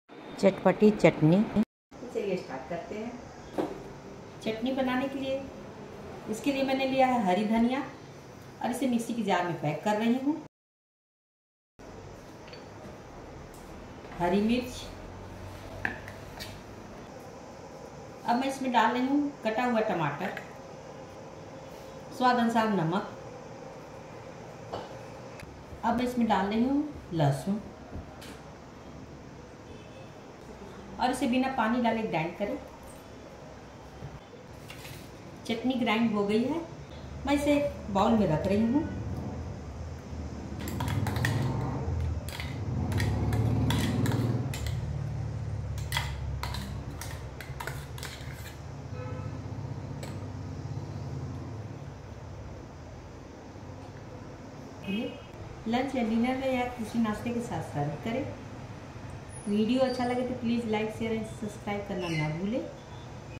तीखी चटपटी चटनी बनाएं। लंच - डिनर या किसी नाश्ते के साथ सर्व करें। चटनी कैसे बनाएं।।
चटपटी चटनी बनाएं। लंच डिनर या किसी नाश्ते के साथ सर्व करें। चटनी कैसे बनाएं।।
आज मैं बनाने जा रही हूं चटनी।
इसके लिए मैंने लिया है।
 हरी धनिया, हरी मिर्च, टमाटर, लहसुन, नमक